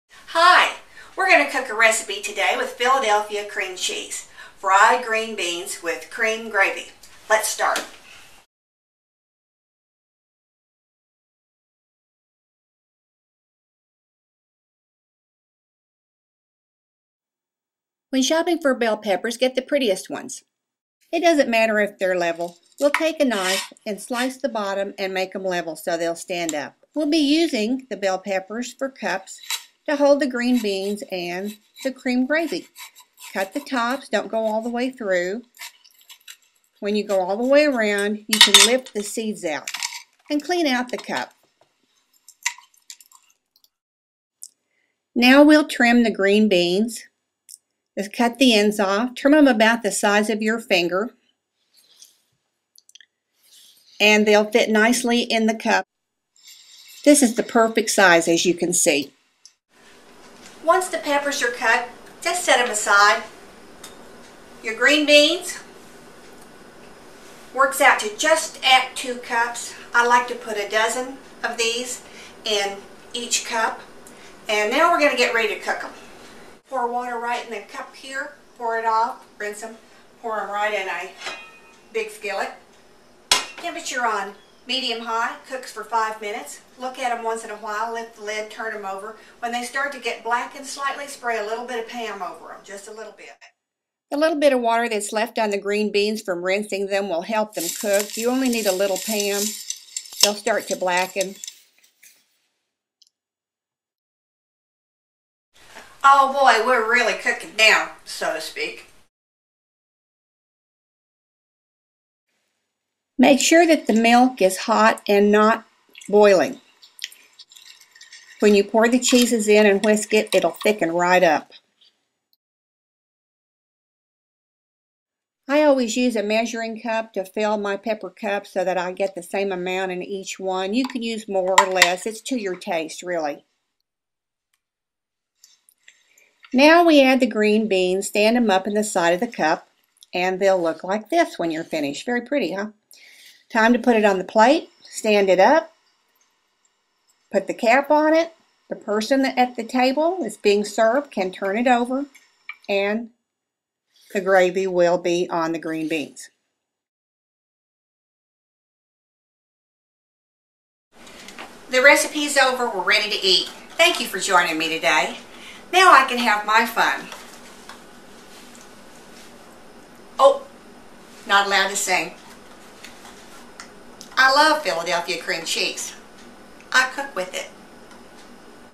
Fried Green Beans with Cream Gravy
Green bean casserole with a twist! Fresh green beans are added to a colorful pepper cup for a beautiful serving idea. Gravy is cheesy and creamy thanks to Philadelphia cream cheese.